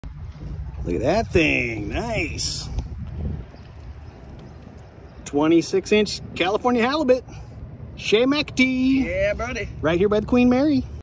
June 9th, 2023 California Halibut next to the World Famous Queen Mary
Yesterday, June 9th, 2023, Shea McIntee, host of the television show STOKED ON FISHING on BALLY SPORTS WEST, went kayak fishing with @OkumaFishingUSA in Long Beach California and caught nice size halibut, spotted bay bass, sand bass, and calico bass